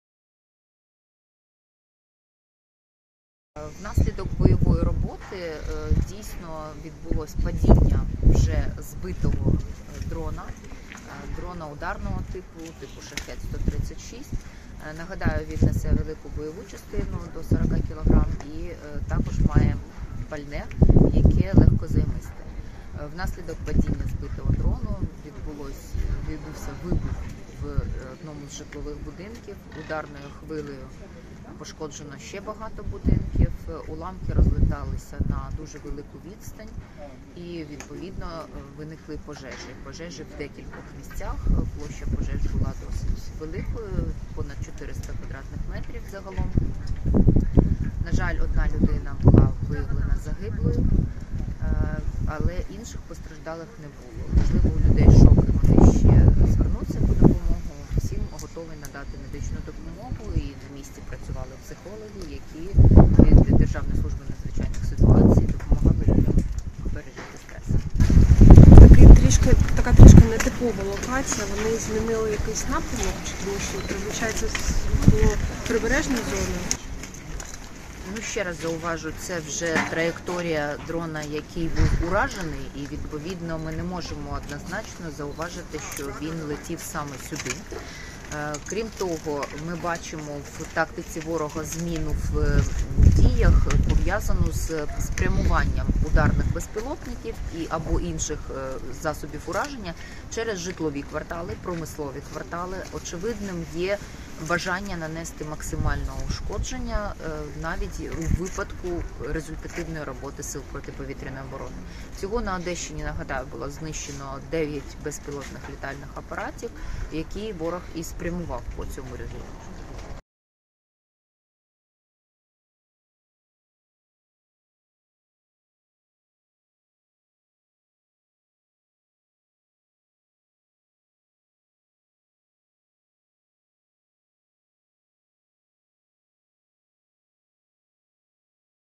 ‼️Понівечені та зруйновані будинки: наслідки атаки дронами Одещини / Наталя Гуменюк | Odesa.LIVE
Черговий обстріл Одеси російськими ракетами приніс нові руйнування, один чоловік загинув. Про це розповіла речниця Оперативного командування "Південь" Наталя Гуменюк.

Джерело відео: ексклюзив редакції Odesa.LIVE